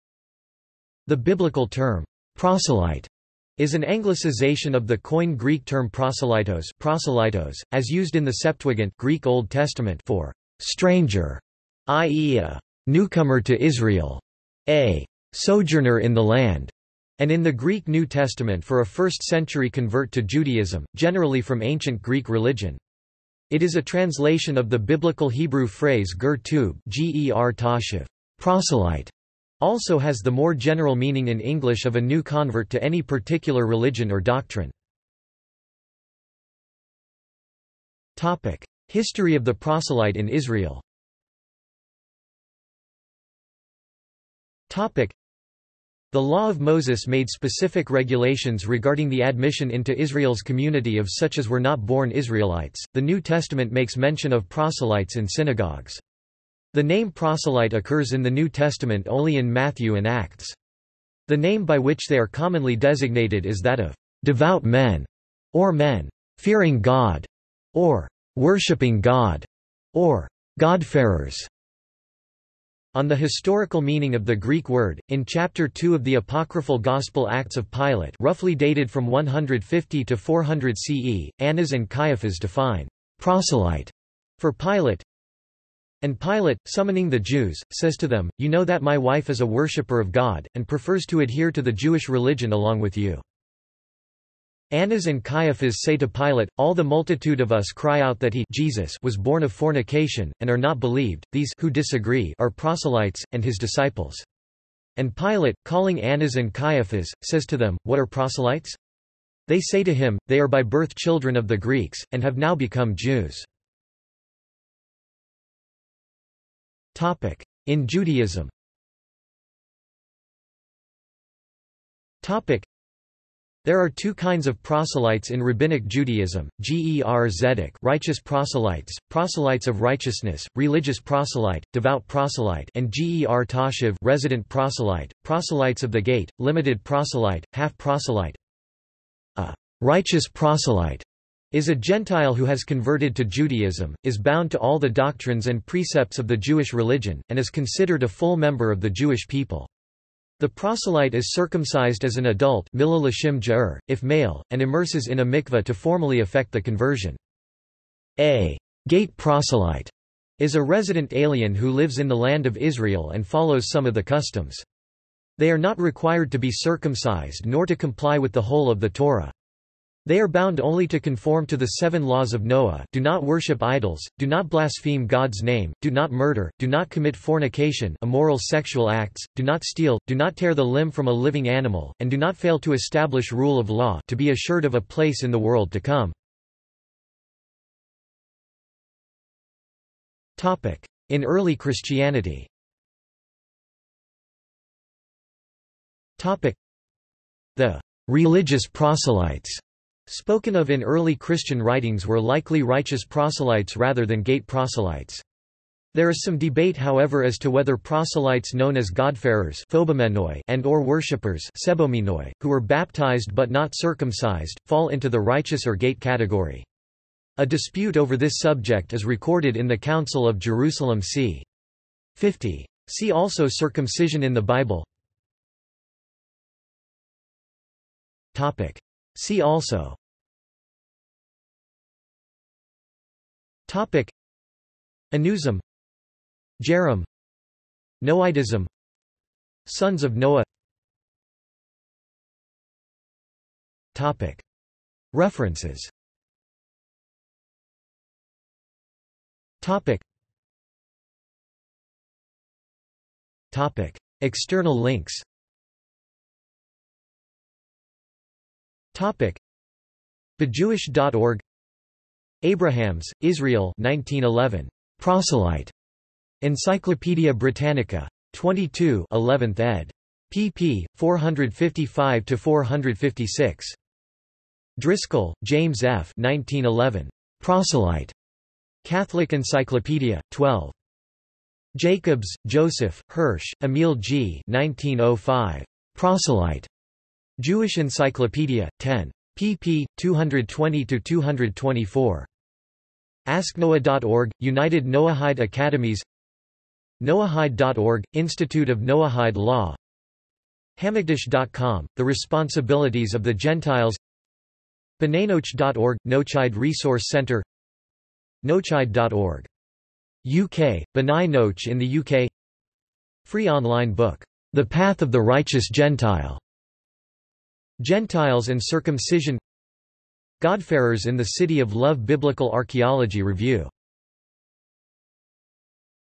Proselyte | Wikipedia audio article
This is an audio version of the Wikipedia Article:
Proselyte

SUMMARY
The biblical term "proselyte" is an anglicization of the Koine Greek term προσήλυτος (proselytos), as used in the Septuagint (Greek Old Testament) for "stranger", i.e. a "newcomer to Israel"; a "sojourner in the land", and in the Greek New Testament for a first-century convert to Judaism, generally from Ancient Greek religion. It is a translation of the Biblical Hebrew phrase גר תושב (ger toshav)."Proselyte" also has the more general meaning in English of a new convert to any particular religion or doctrine.